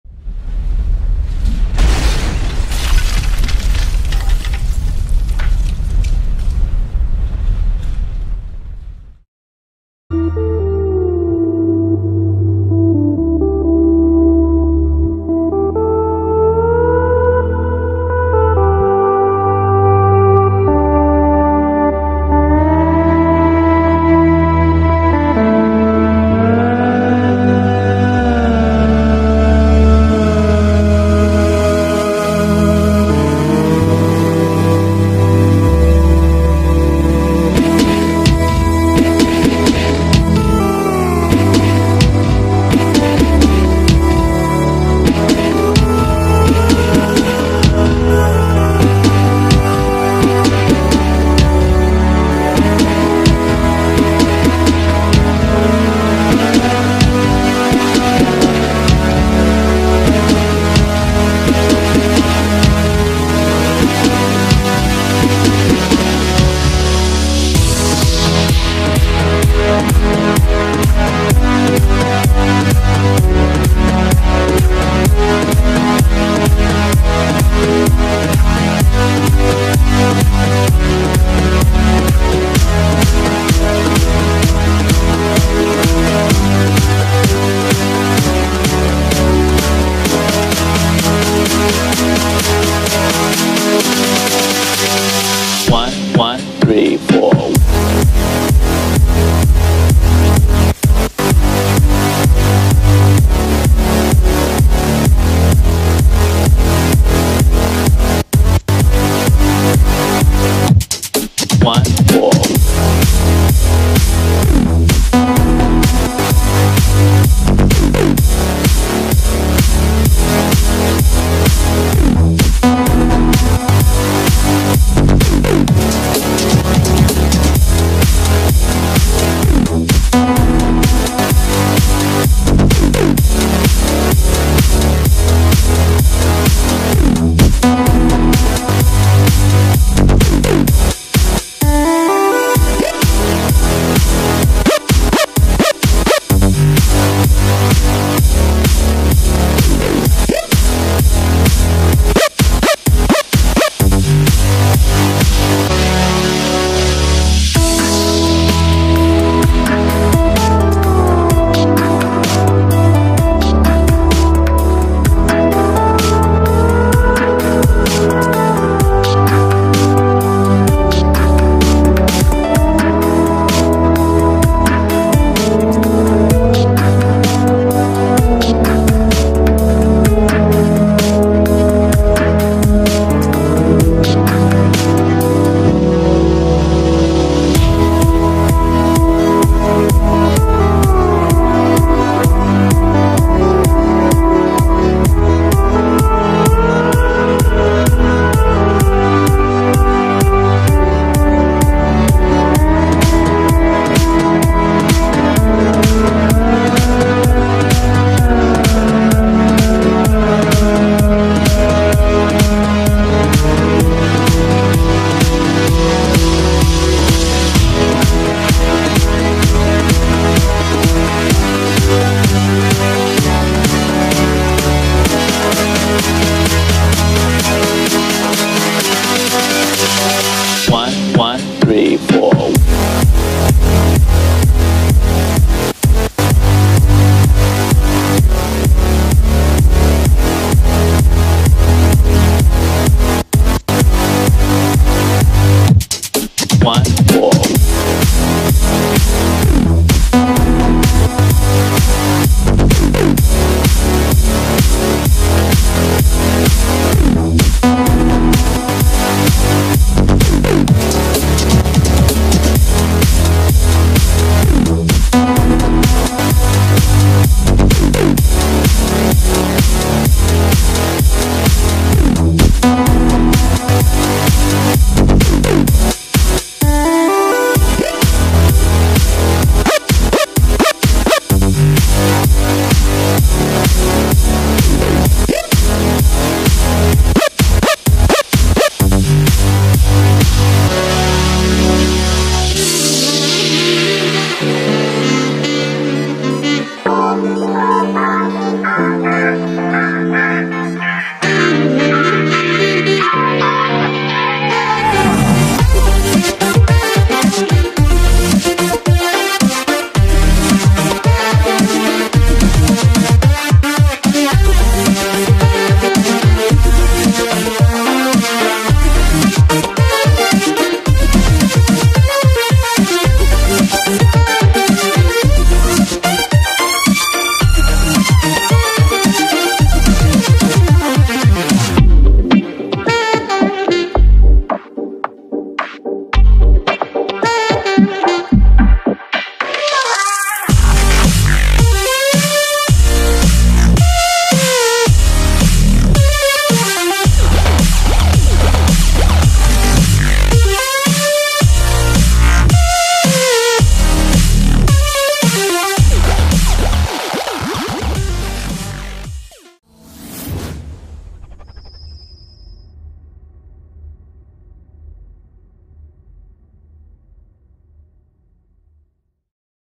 EVIL MINNION ! scrap metal sculpture time lapse welding build.
This is another of my time lapse builds... now for sale on ebay.